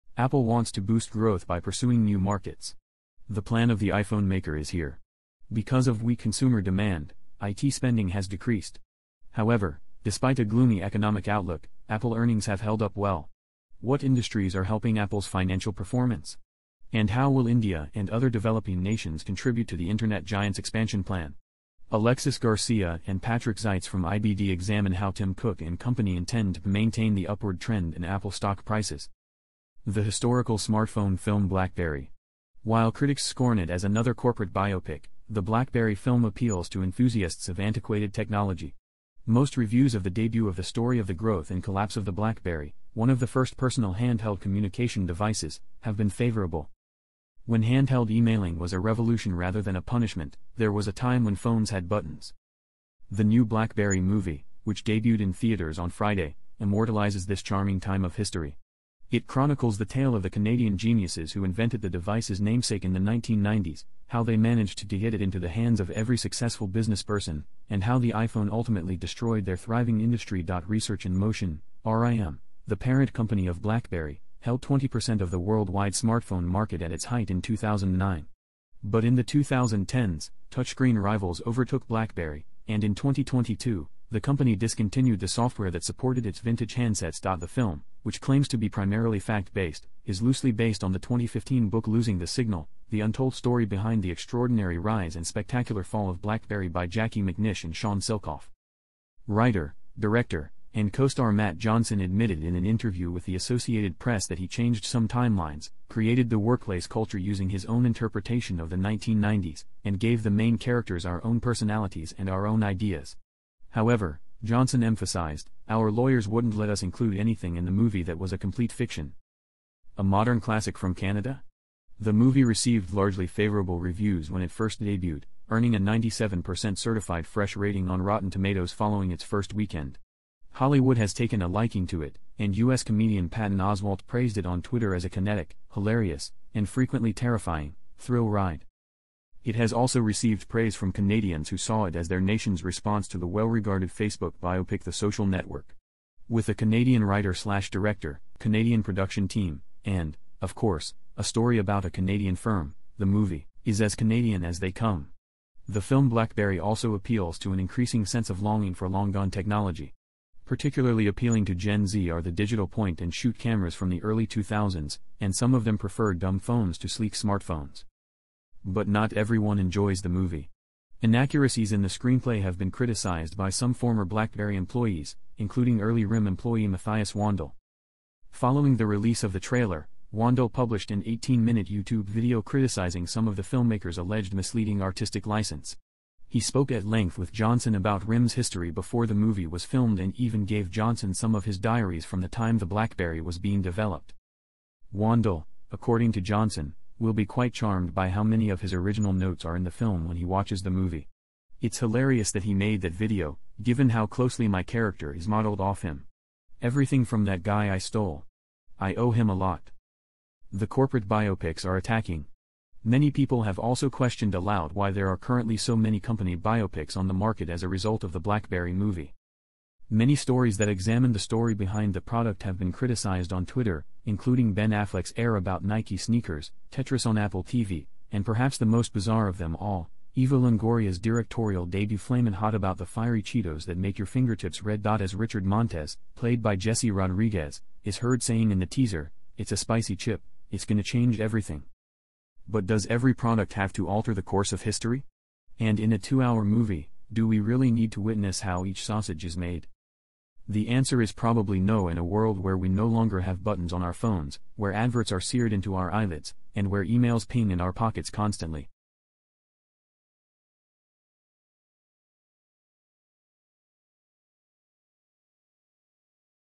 Apple wants to boost growth by pursuing new
Apple wants to boost growth by pursuing new markets. The Plan of The iPhone Maker Is Here.
Because of weak consumer demand, IT spending has decreased. However, despite a gloomy economic outlook, Apple earnings have held up well. What industries are helping Apple's financial performance? And how will India and other developing nations contribute to the internet giant's expansion plan? Alexis Garcia and Patrick Seitz from IBD examine how Tim Cook and company intend to maintain the upward trend in Apple stock prices.